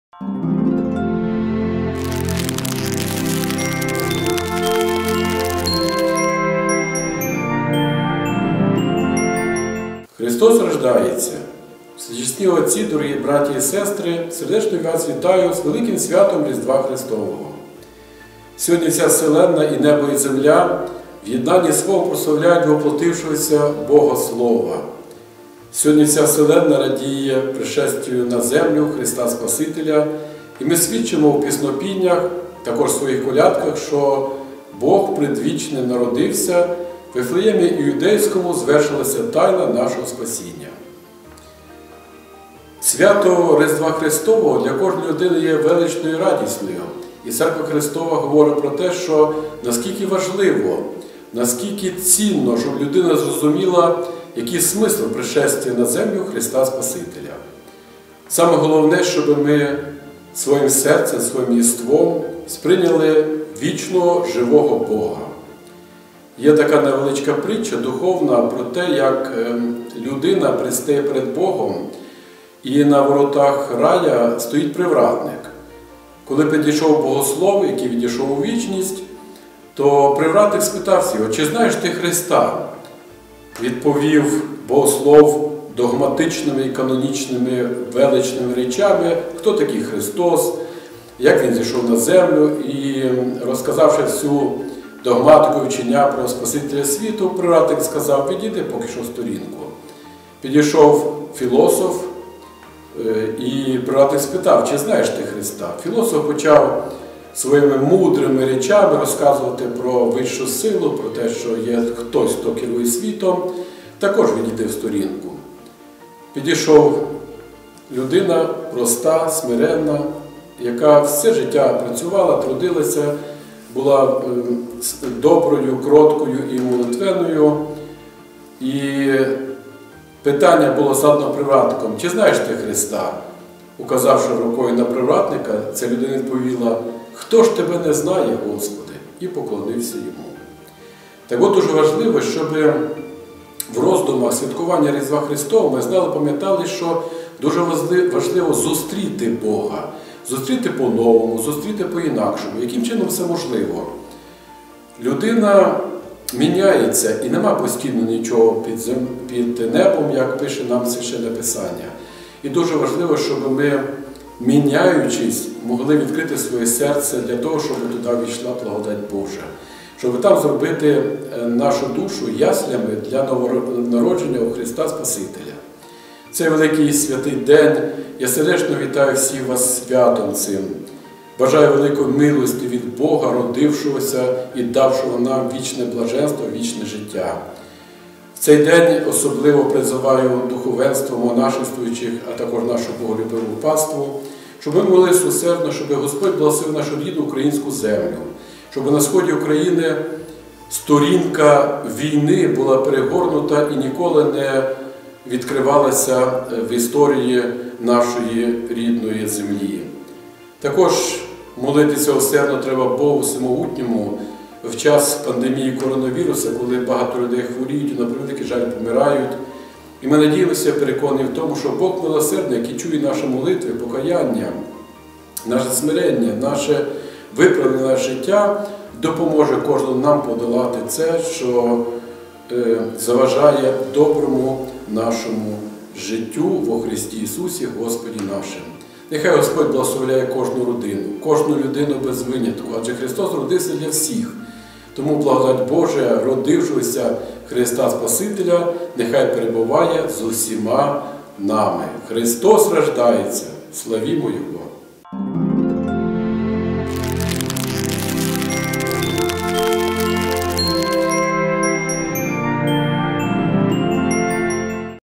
Привітання від Феодора
Феодор - Митрополит Мукачівський і Ужгородський щиро вітає закарпатців з Новим роком та Різдвом Христовим!